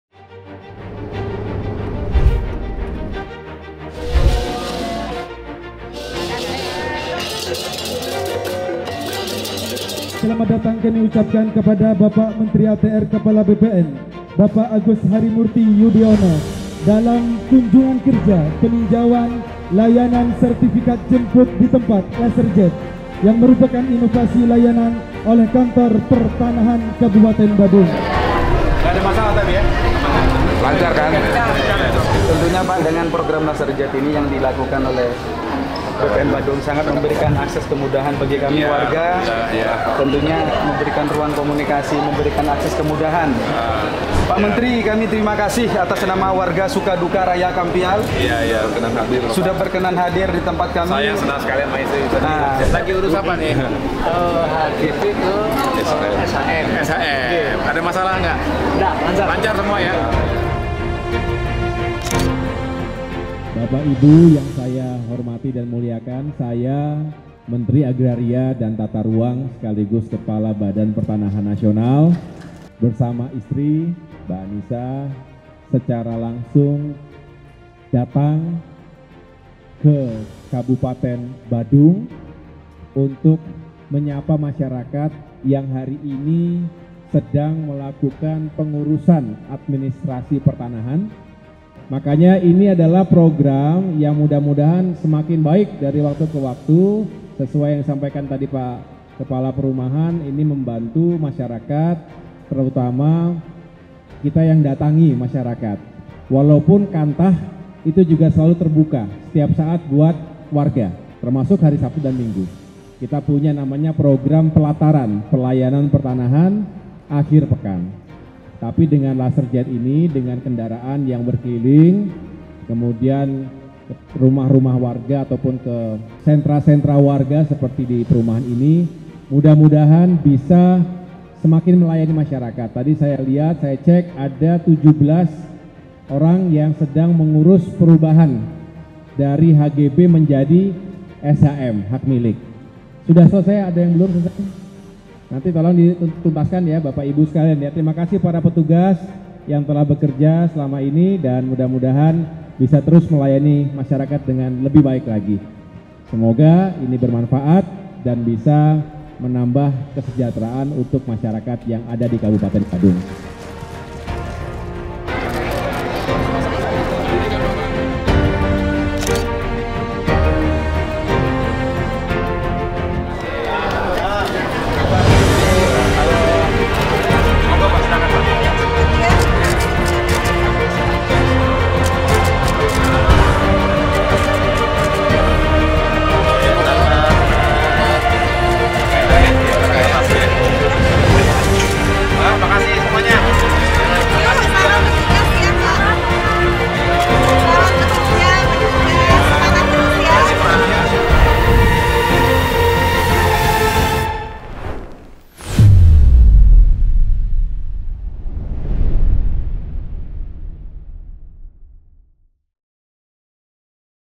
Meninjau Layanan Sertipikat Jemput di Tempat (Laser Jet) oleh Kantah Kab. Badung
Dengan Laserjet (Layanan Sertipikat Jemput di Tempat) layanan pertanahan jauh lebih cepat karena bisa langsung diurus dekat dengan tempat tinggal masyarakat sehingga tidak perlu jauh-jauh datang ke Kantah.

Ditemani Annisa saya mengecek langsung layanan tersebut di Kab. Badung, Bali. Masyarakat juga menilai bahwa dengan ini pengurusan tanah tidak lagi memerlukan calo dan betul-betul efisien.

Terima kasih dan apresiasi kepada Kanwil dan Kantah yang telah memberikan layanan terbaik bagi masyarakat!